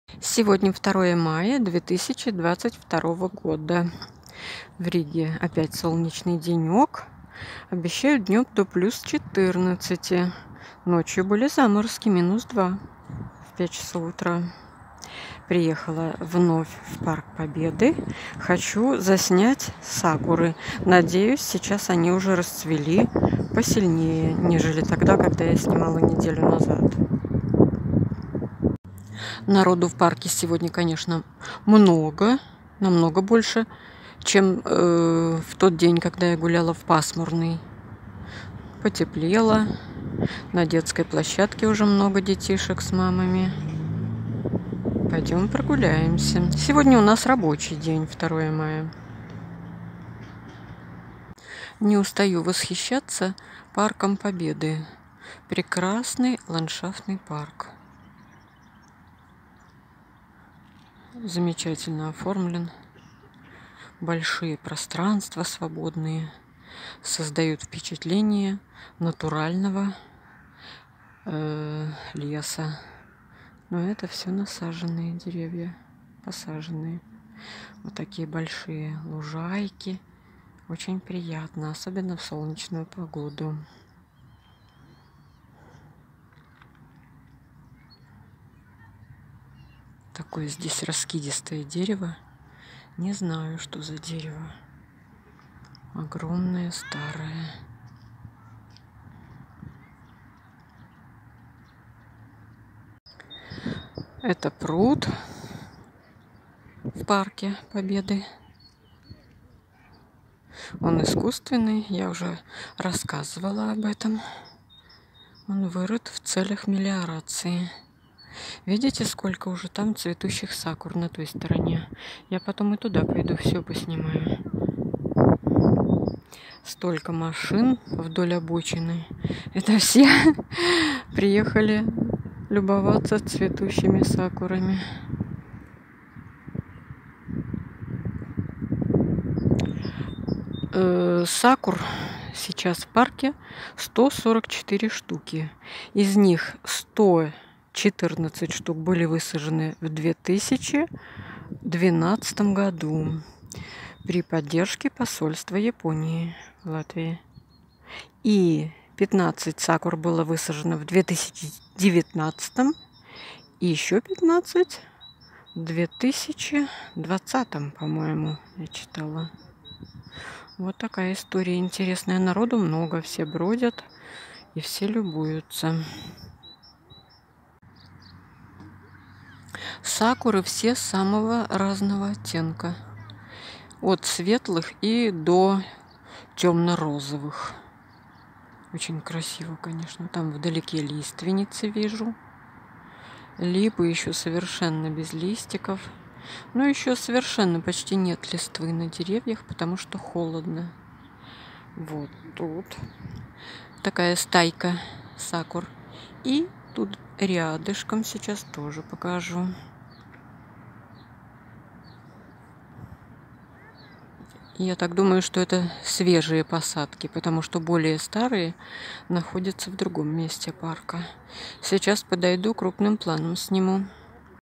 Рига.И вновь сакура.Часть 1-я.
Рига,парк Узварас,2 мая 2022 года.